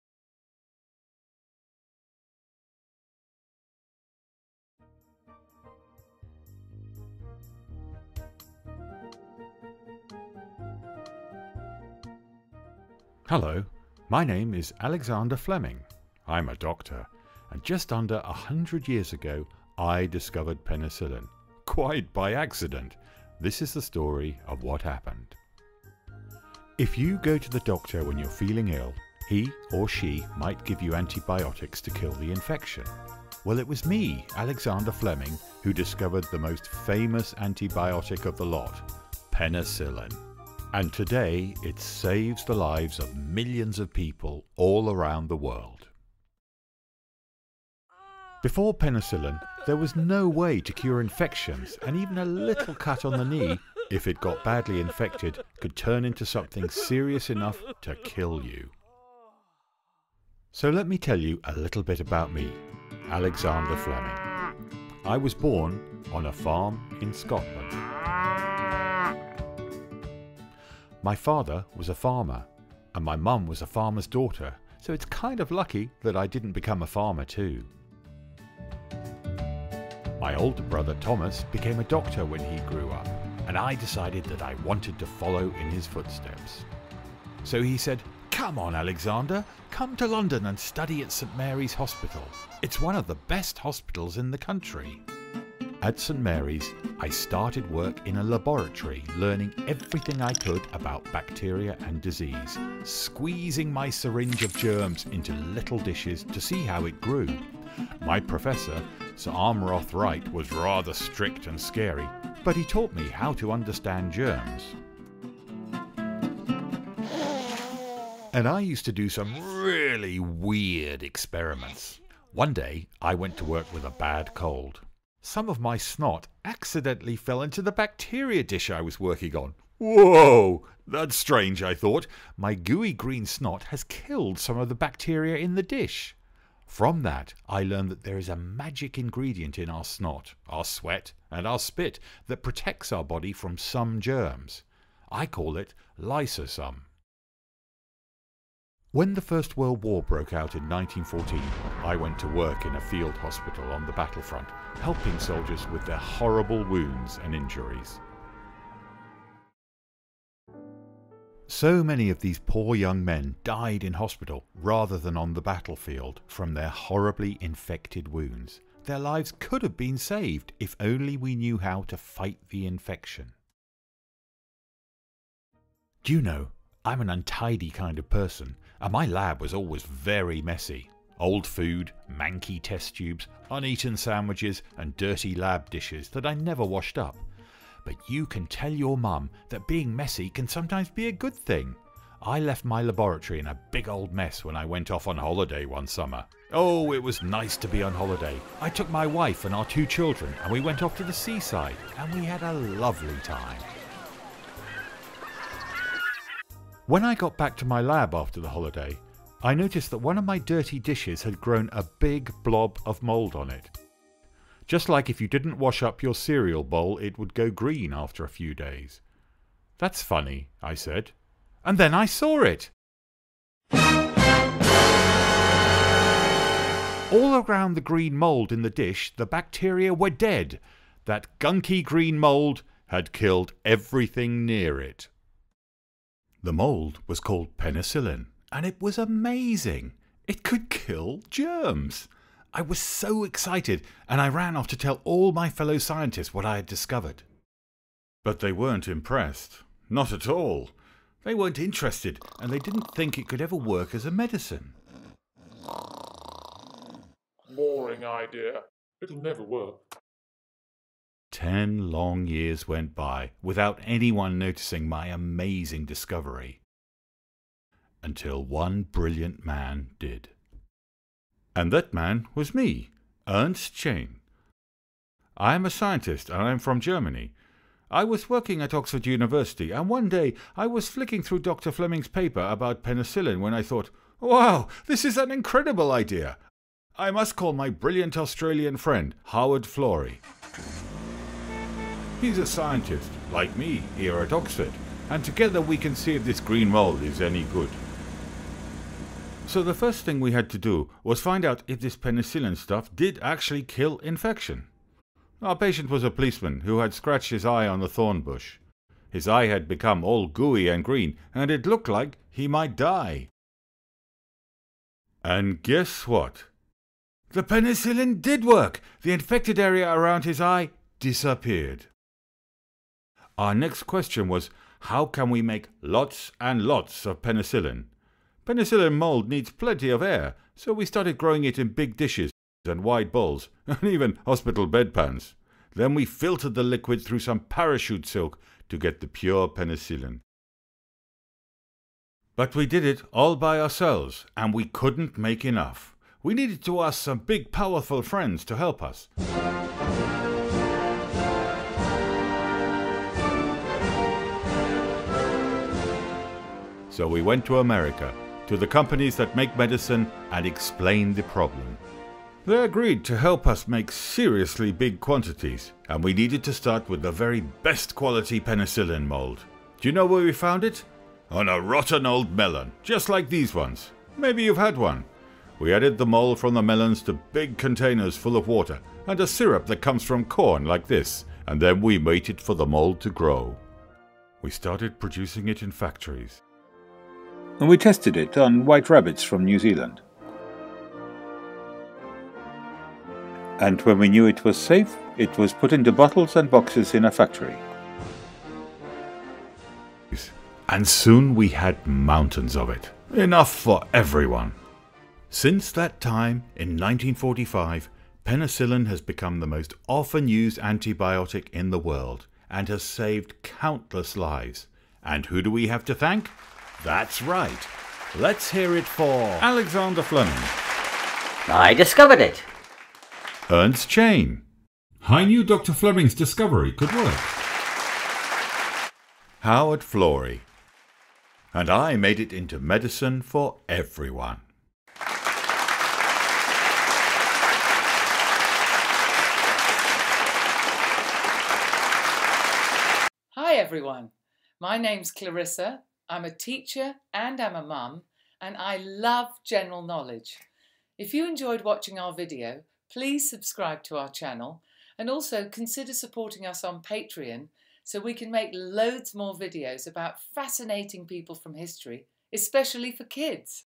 Alexander Fleming and the discovery of penicillin
Check out the life of great Scottish scientist Alexander Fleming and find out exactly how he stumbled on the antibiotic penicillin.  Don't forget the clever people who took his discovery and made it into medicine that saves lives all around the world.